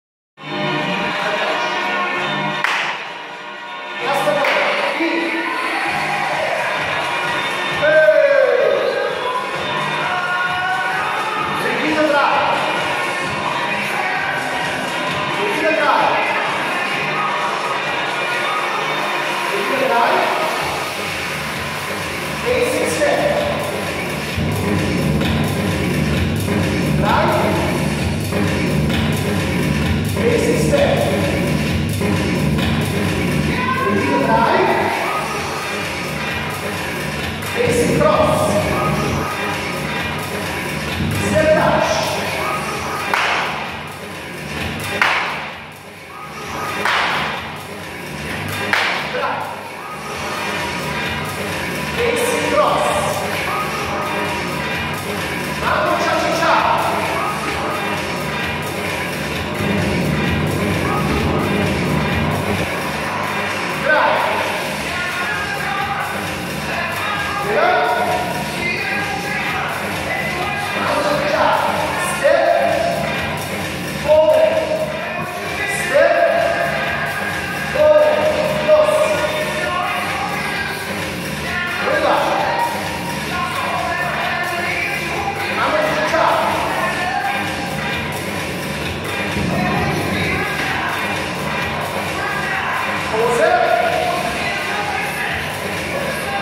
Basic Step Aerobic with Daniel Bata 1601
Hier der Aufbau des ersten Blocks meiner Basic Step Aerobic Stunde